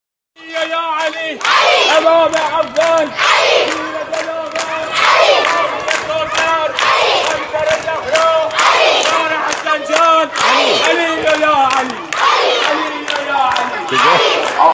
مدارس إیران
فیدیو ... ‌ملا مهرج في مدارس إیران !!
ثقافة الملالي للأطفال بهدف غسيل الدماغ !!
کما یفعلونها فی الیمن وسوریا و. لأجل أن یجلعوا منهم وقود
حربهم التوسعیة في المنطقة .. وضد الشعب الإیراني